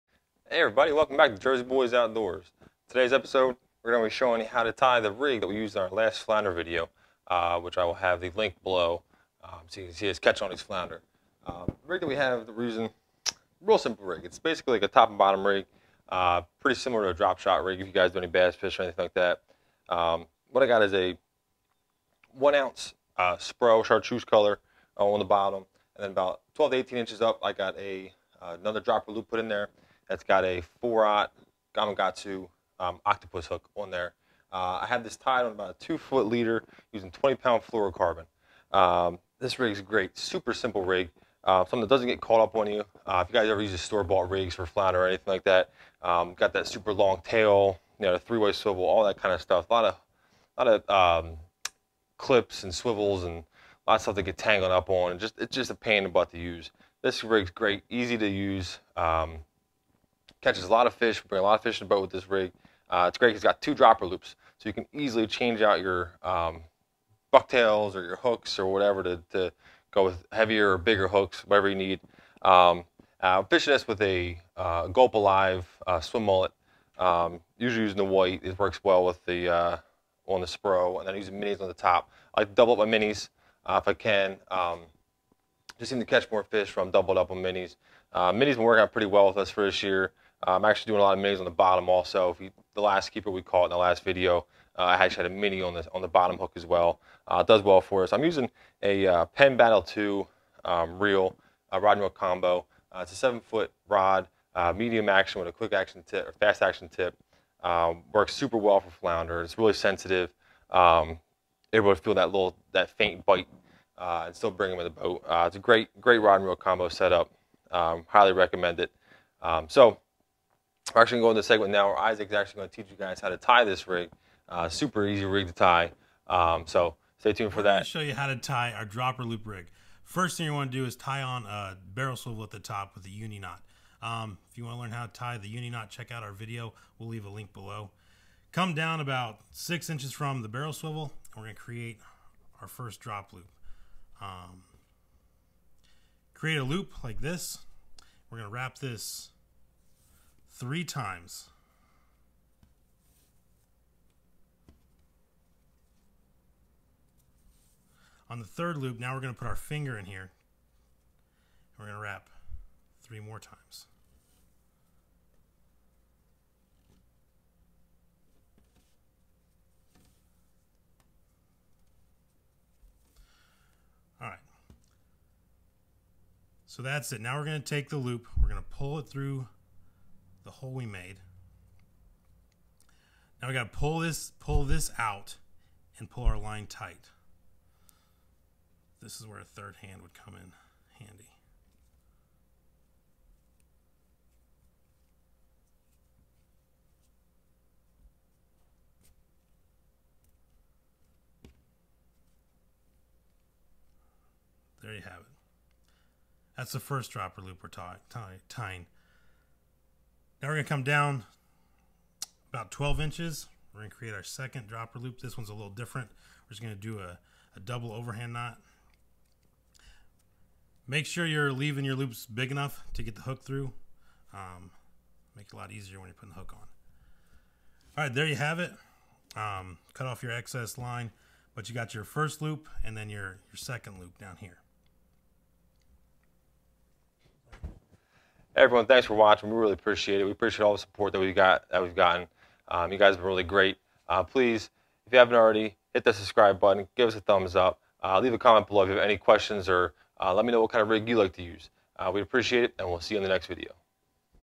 The Best Flounder Rig
This is a super simple rig that will land you a ton of flounder.

Gear:
ROD AND REEL